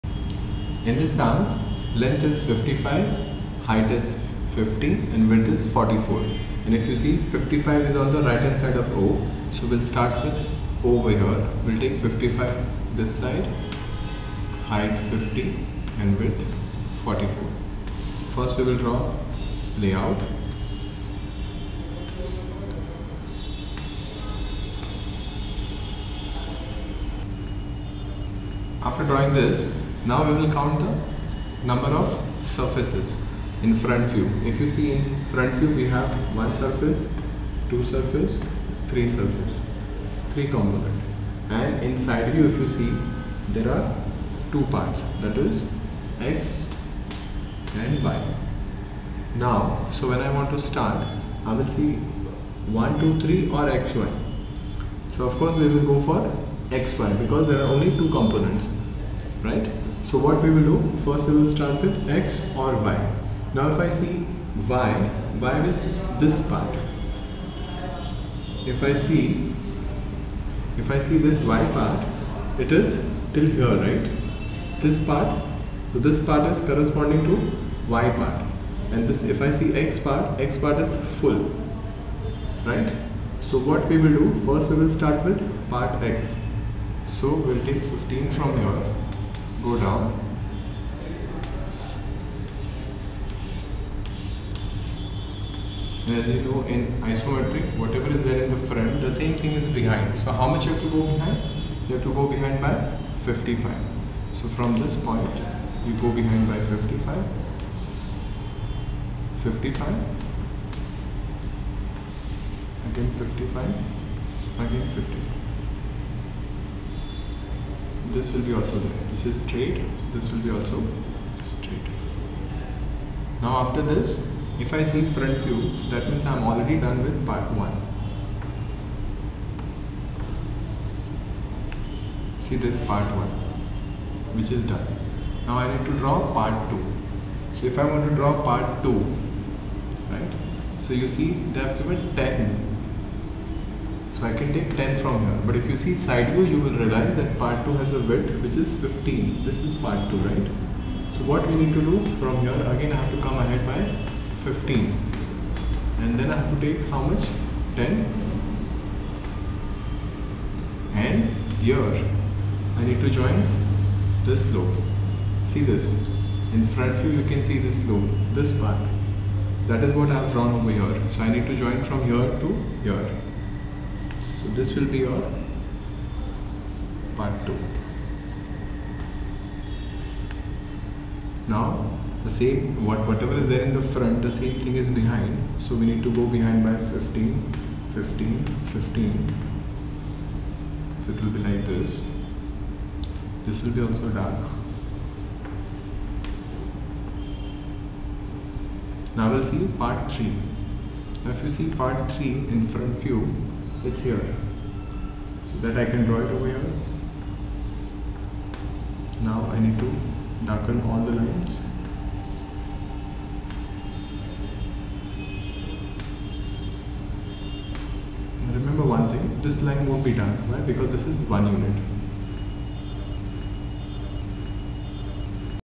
Isometric view Question 12
Converting Two dimensional (2D) Figure into Three dimensional Figure (3D).